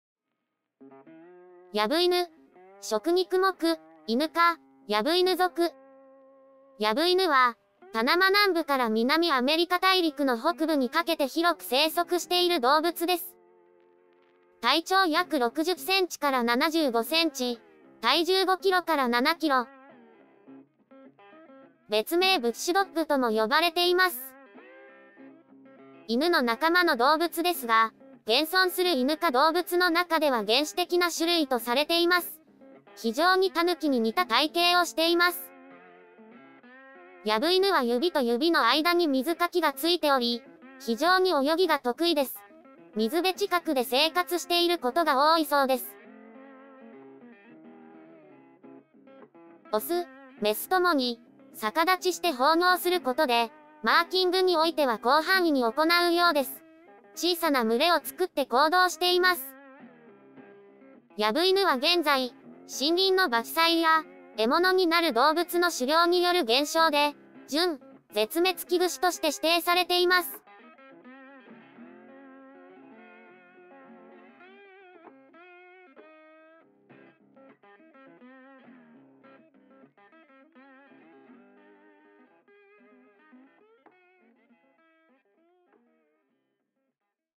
ヤブイヌ：東山動物園
日本国内では６カ所で飼育されています。犬の仲間において、非常に原始的な形をしています。泳ぎが得意で、足には水かきがついています。
ボールを巣に持っていく場面が、撮影できました。冬に何度か観察しましたが、あまり外に出ていないために撮影には苦労しました。